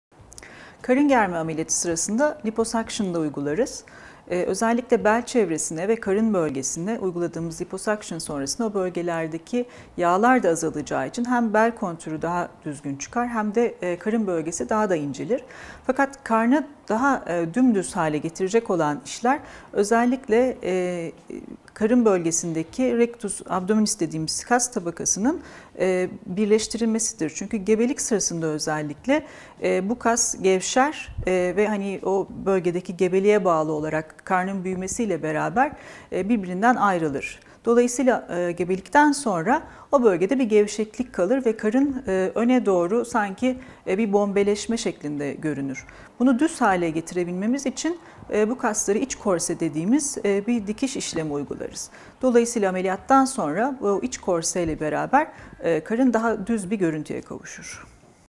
Karın germe operasyonu sonrası karın düzleşir mi? Dr. Leyla Arvas anlatıyor.
Harbiye Mah. Abdi İpekçi Cad. Park No:19-1 Kat:2
Nişantaşı-Şişli-İstanbul-Türkiye 34367